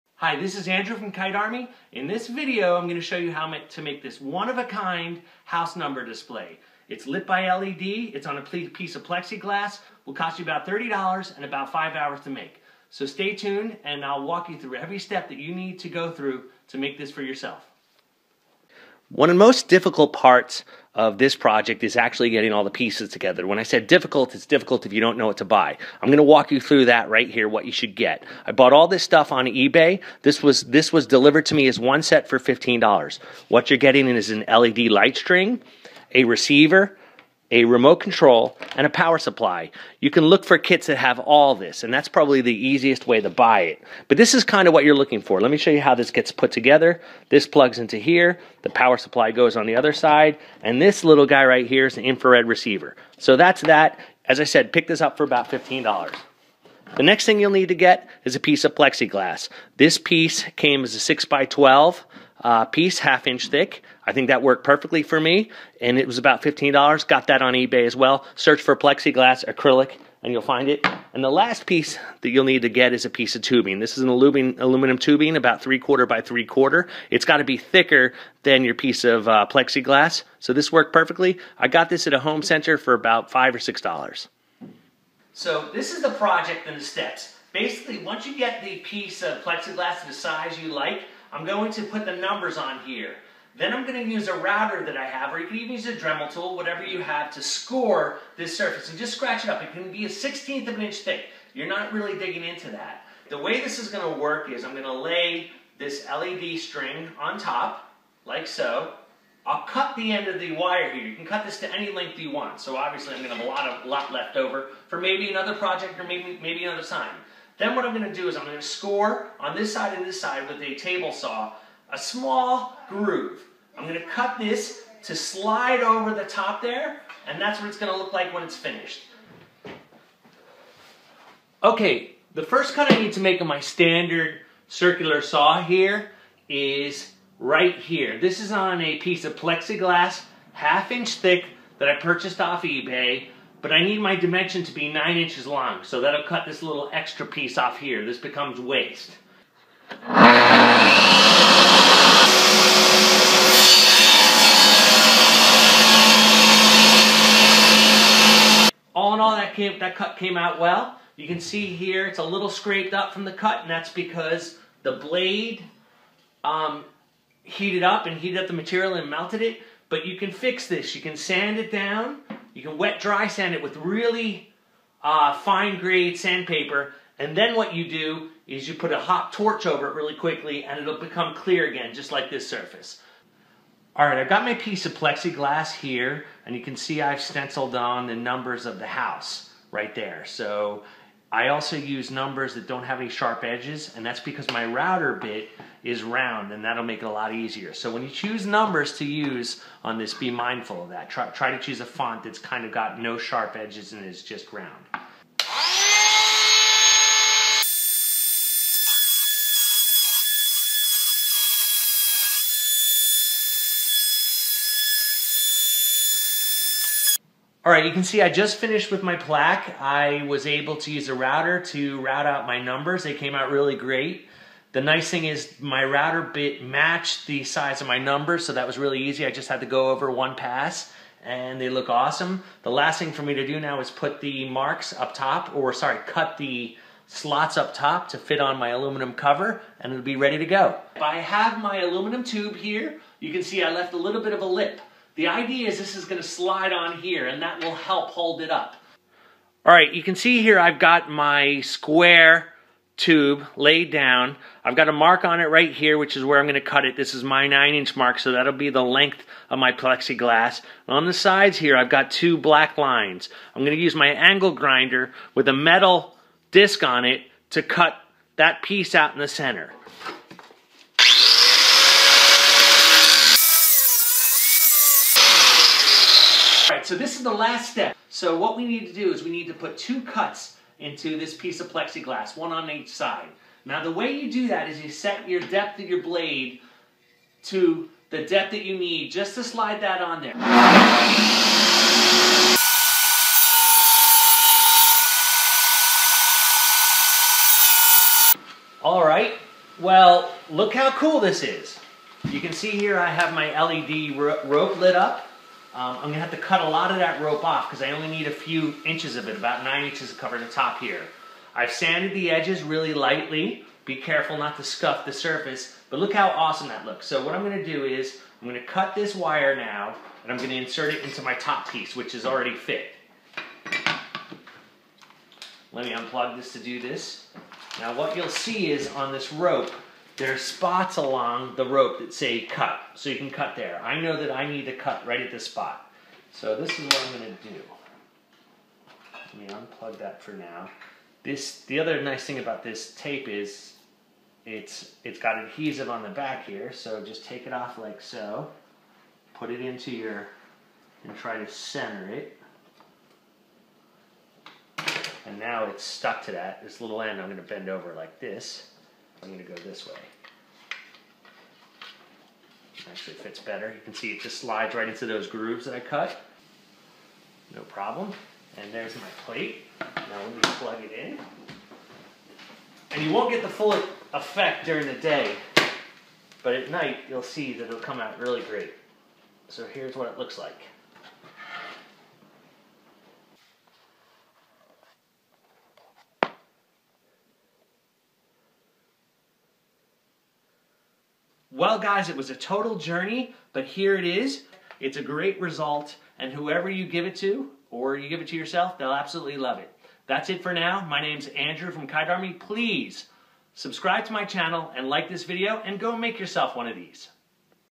Make LED Light House Number - DIY Build Project
AMAZON PRIME DISCOUNT LINK (best deal)

Build this one-of-a-kind LED house number for $30 in under 4 hours.  SO GO AND GET STARTED!